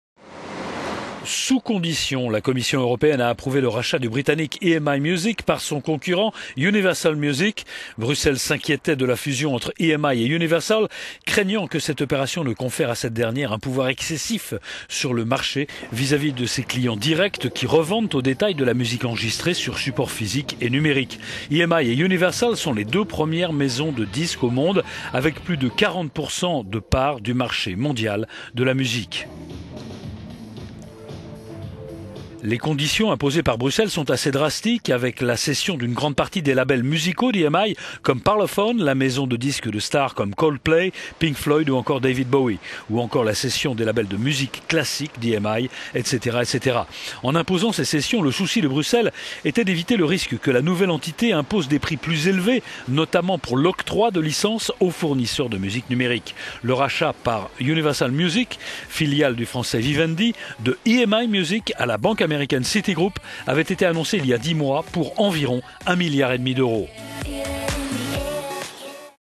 Universal Music va pouvoir racheter EMI sous conditions
Sous conditions, la Commision européenne a approuvé le rachat du britannique EMI Music par son concurrent Universal Music. Bruxelles s'inquiétait de la fusion entre EMI et Universal craignant que cette opération ne confère à cette dernière un pouvoir excessif sur le marché vis-à-vis de ses clients directs qui revendent au détail d ela musique enregistrée sur support physique et numérique. EMI et Universal sont les deux premières maisons de disques au monde avec plus de 40% de part du marché mondial de la musique.

Les conditions imposées par Bruxelles sont assez drastiques : avec la cession d'une grande partie des labels musicaux d'EMI comme Parlophone, la maison de disques de stars comme Coldplay, Pink Floyd ou encore David Bowie; ou encore la cession des labels de musique classique d'EMI. etc...etc.

En imposant ces cessions, le souci de Bruxelles  était d'éviter le risque que la nouvelle entité impose des prix plus élevés notamment pour l'octroi de licences aux fournisseurs de musique numérique.

Le rachat par Universal Music, filiale du français Vivendi de EMI Music à la banque américaine Citigroup avait été annoncé il y a 10 mois pour environ 1,5 milliard d'euros.